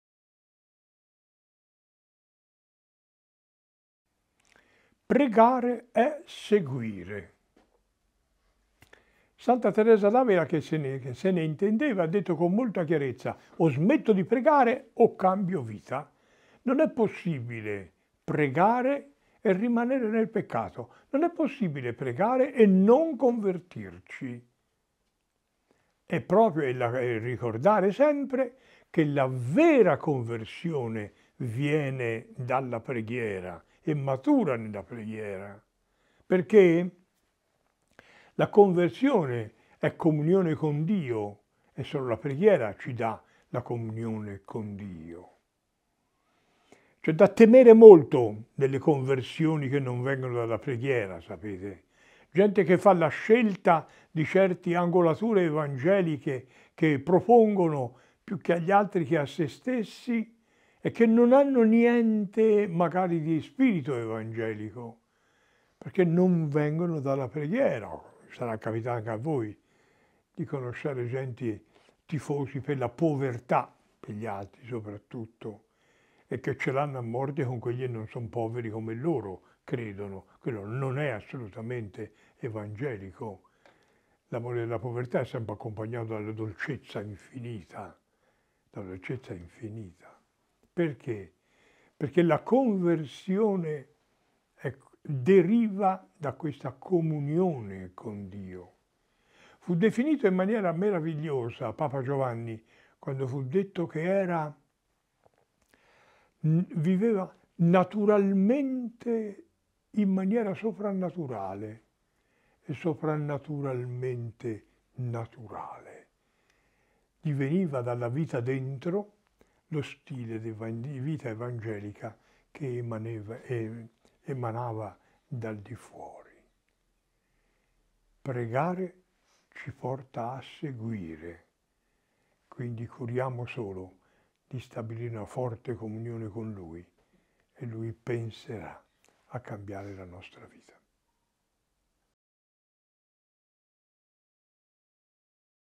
Pregare è seguire
Santa Teresa di Avila diceva “ O smetto di pregare o cambio vita” diceva così la dinamica della vera conversione.

La preghiera è comunione con Dio , se è vera comunione col Signore necessariamente ci mettiamo alla sua sequela.

La Chiesa ci presenta il Cristo Pasquale con l’immagine del Pastore Bello che le pecore ascoltano e seguono, anzi, dialogano con Lui, perché riconoscono la sua voce e Lui il loro belato.

Se alla preghiera non corrisponde un cambiamento di vita la preghiera può diventare una pura ginnastica spirituale e anche se diciamo molte formule, direbbe San Benedetto, la lingua “Frustra laborat” lavora invano.

E’ importante notare come la conversione vera è quella che è conseguenza di preghiera di comunione con Dio. Non è infatti concepibile cambiare comportamento senza cambiare interiormente  condizione di vita , di sentimenti e di pensiero. Un cambiamento di condotta, anche in direzione evangelica che non derivi da una esperienza di preghiera è sempre pericoloso come quelli che scelgono la povertà evangelica non per amore di Cristo ma per odio di chi non è povero. Il cambiamento di vita, autentica conversione, avviene sempre in modo naturale, naturalmente. Il santo vero deve essere, come fu detto di Papa Giovanni, Naturalmente soprannaturale e soprannaturale naturalmente.

La qualità della preghiera si vede dalla qualità della vita evangelica della persona. Santa Teresa di Avila nel suo capolavoro, il Castello interiore, fa coincidere i livelli di preghiera con i gradi si santità.